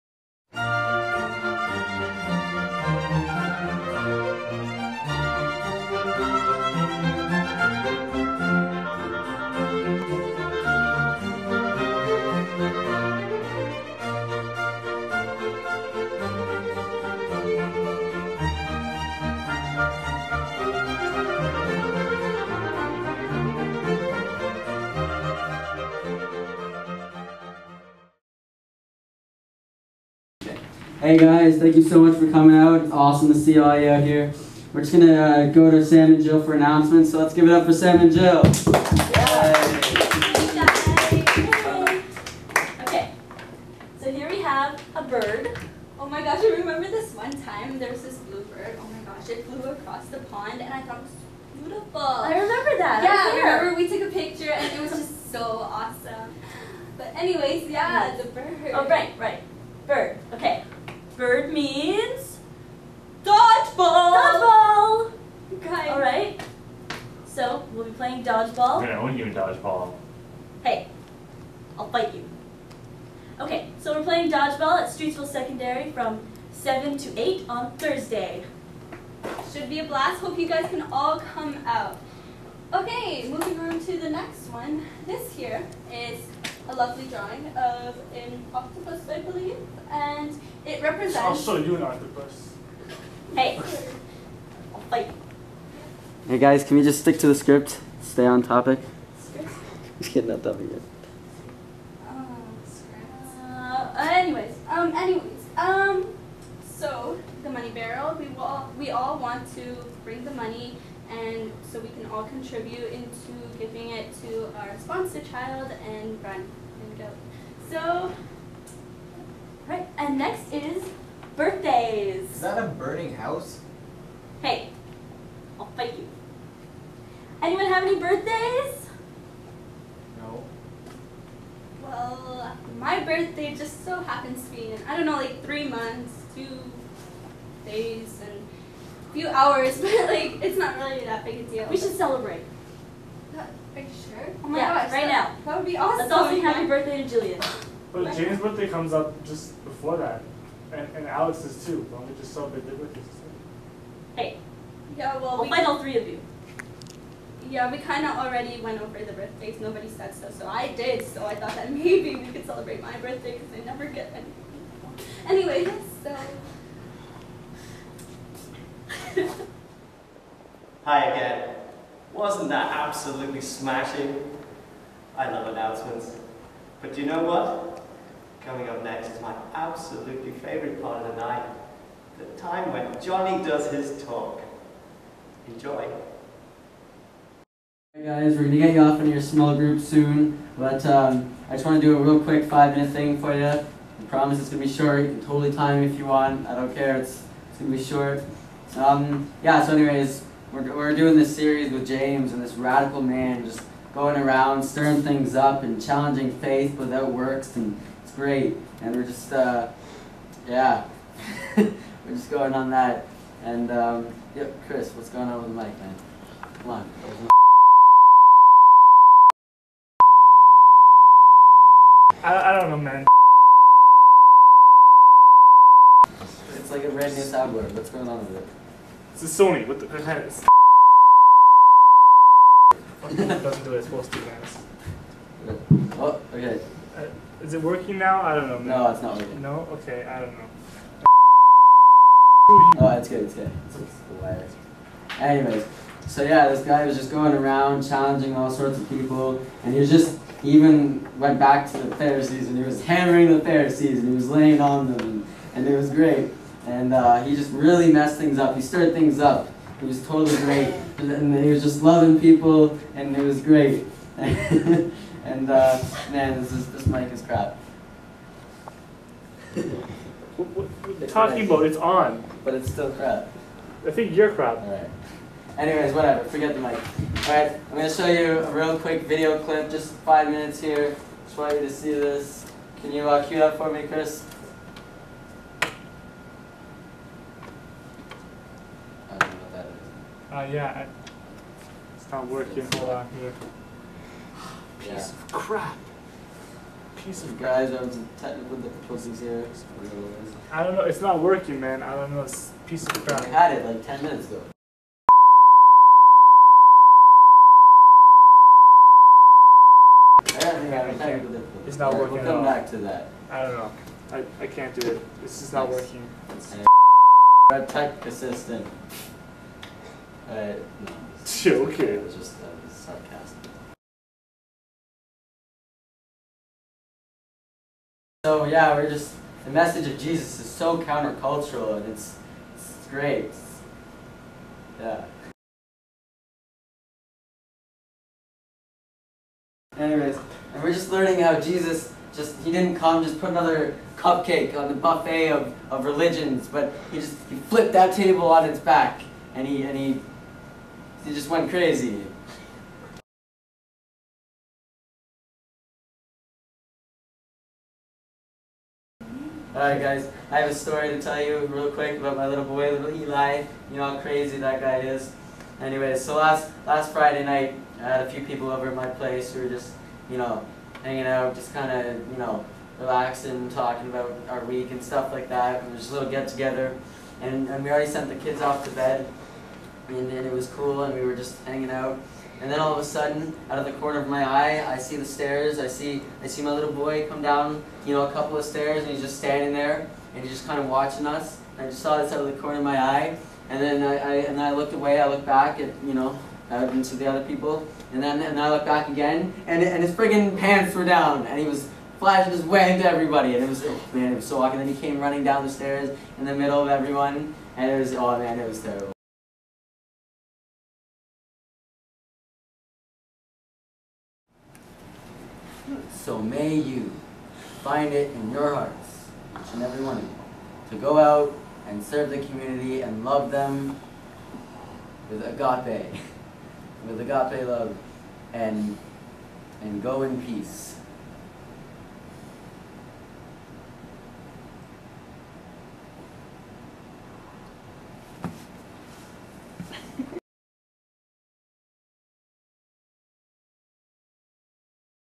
UTI Mockumentary Part 1
The ultimate behind-the-scenes look at what happens at UTI. This clip includes announcements and Jon's talk.